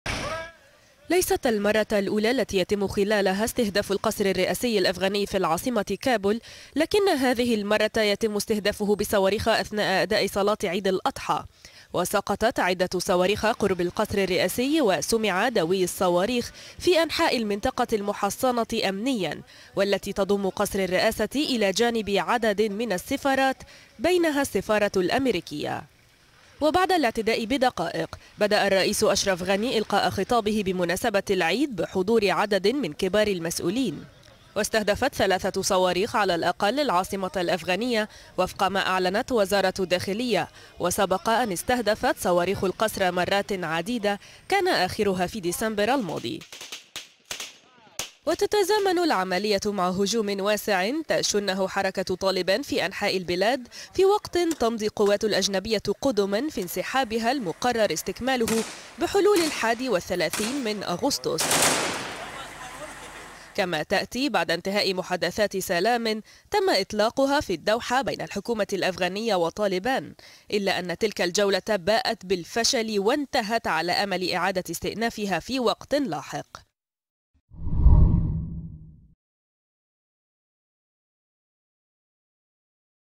أفغانستان | سقوط صواريخ قرب قصر الرئاسة في كابول خلال صلاة عيد الأضحى
سقطت عدة صواريخ قرب القصر الرئاسي الأفغاني في العاصمة كابول، خلال صلاة عيد الأضحى ، وسمع دوي الصواريخ في أنحاء المنطقة المحصّنة أمنيا والتي تضم قصر الرئاسة إلى جانب عدد من السفارات، بينها السفارة الأمريكية... المزيد من التفاصيل عبر التقرير المصور.